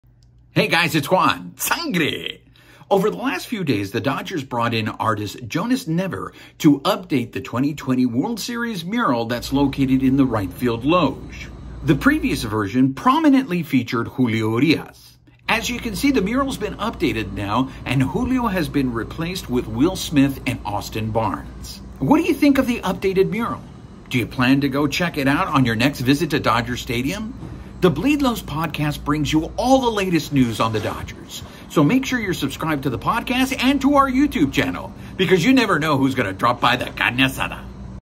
Dodger Stadium updates World Series Somos Los Dodgers mural that featured Julio Urías
Dodger Stadium updates World Series Somos Los Dodgers mural that featured Julio Urías and now features Will Smith and Austin Barnes